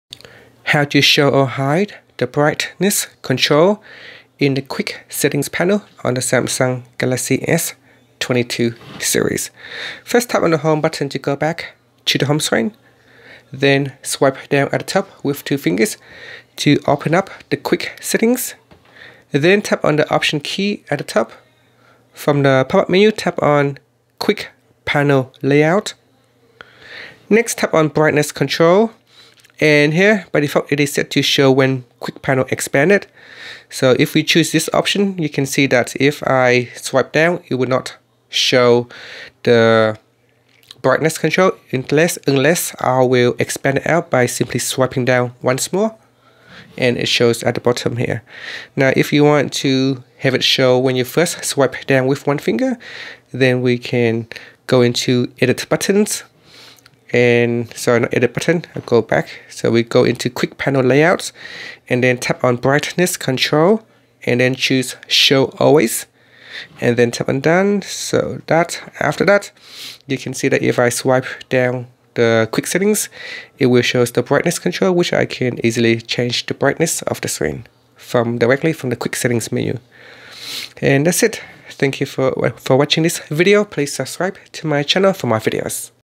Galaxy S22/S22+/Ultra: How to Show/Hide Brightness Control In Quick Settings Panel
Learn how you can show/hide brightness control in quick settings panel on the Samsung Galaxy S22 / S22+ / S22 Ultra.